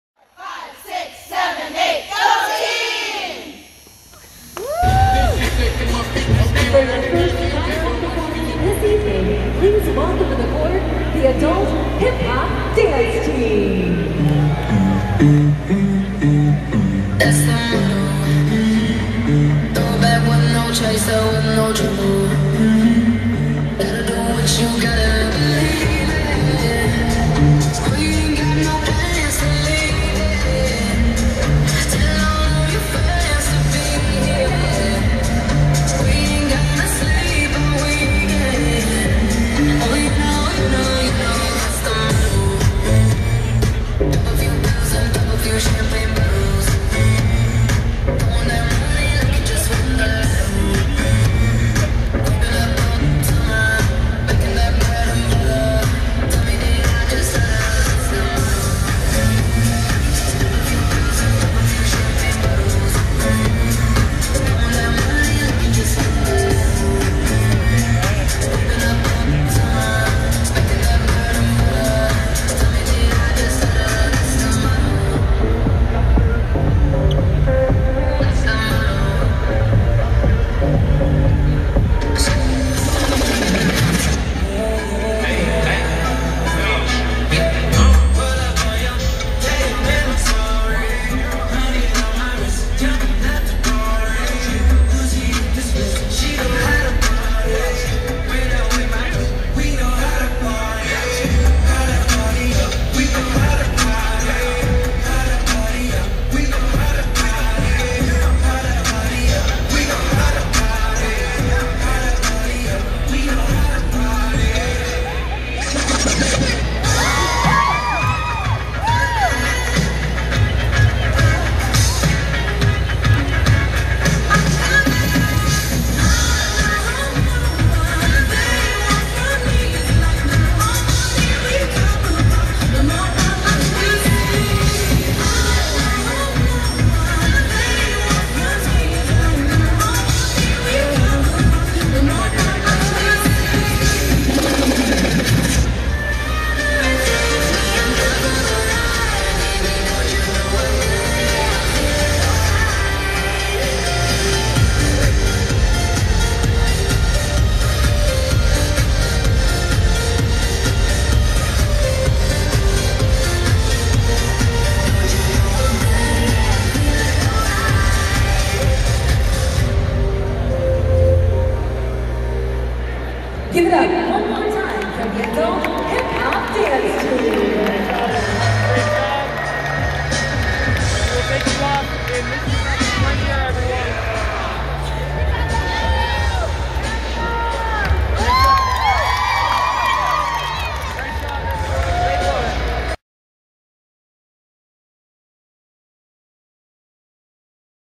Cleveland Cavaliers Dance Performance | The Adult Hip Hop Dance Team
The Adult Hip Hop Dance Team performed live at the biggest Cleveland Cavaliers Game of the season! (against the Golden State Warriors)

Here is a video showcasing their amazing performance at Rocket Mortgage FieldHouse!

Wanna join this team? We are now taking new members! Just contact the team's Creator/Director/Choreographer Ryan Rosinski